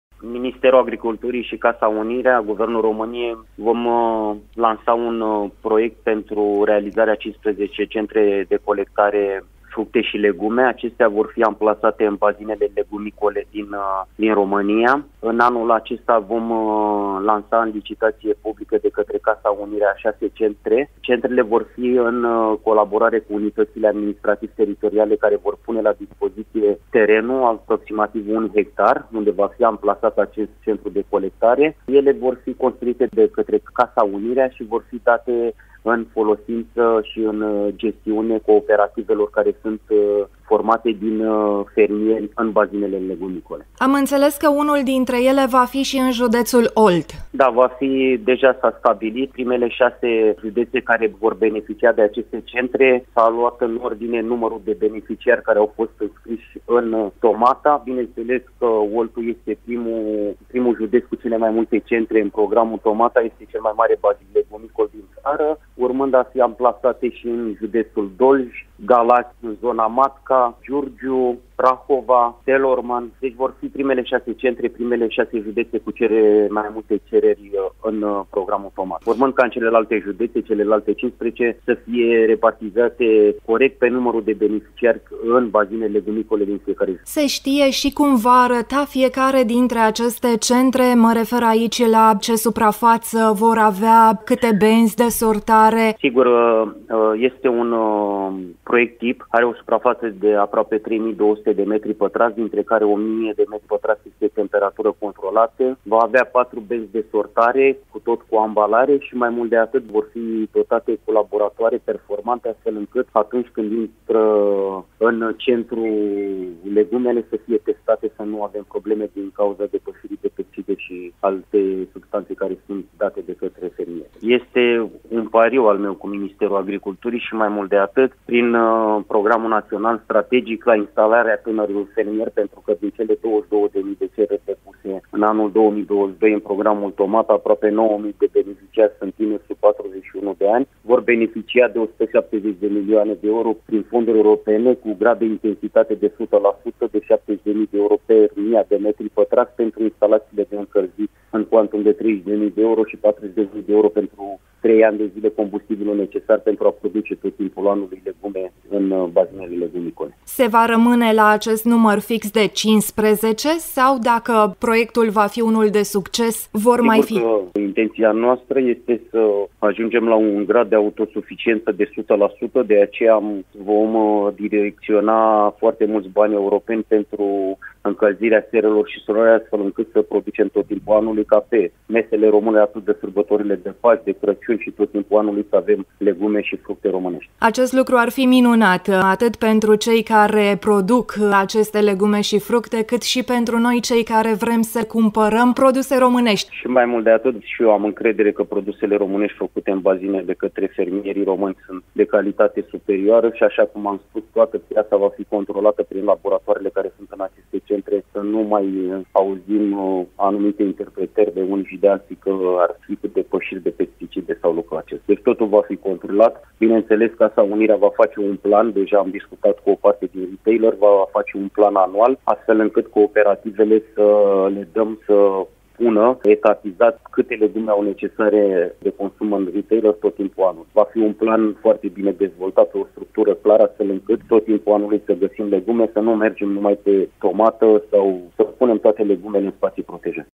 Legume și fructe romanesti! Ministrul agriculturii Florin Barbu! #2023
Șase centre de colectare legume-fructe se vor construi anul acesta,unul în județul Olt! Ministrul agriculturii Florin Barbu!